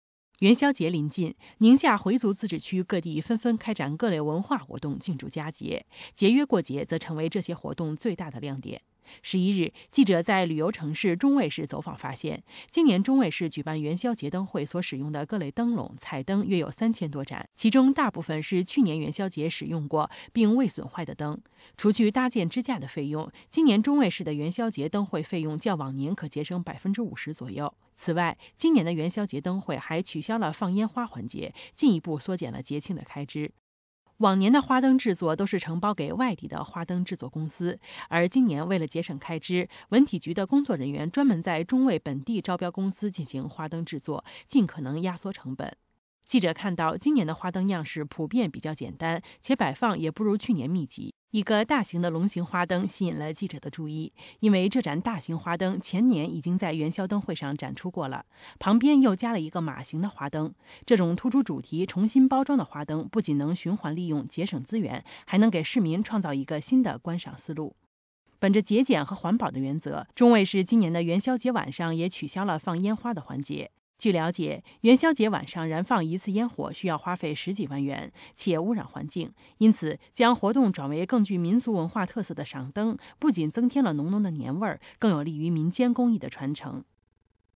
新華社》寧夏中衛：節儉風下花燈更「接地氣」
元宵節臨近，寧夏回族自治區各地紛紛開展各類文化活動慶祝佳節，今年的花燈樣式普遍比較簡單，且擺放也不如去年密集。